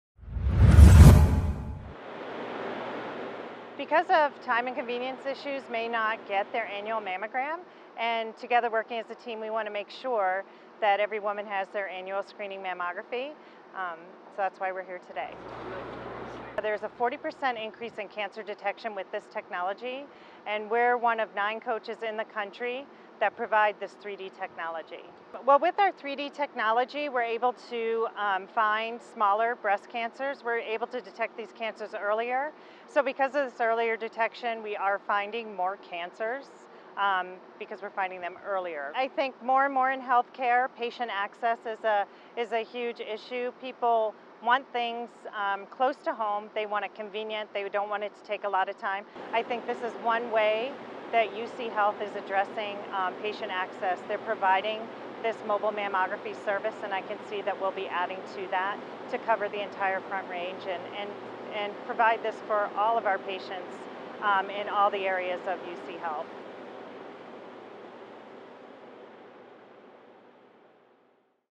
Pink Life Saver in downtown Colorado Springs
The only mobile mammography coach in Colorado with 3-D Diagnostic capability made a trip to Acacia Park.  It was here for mammogram screenings for women 40 and older.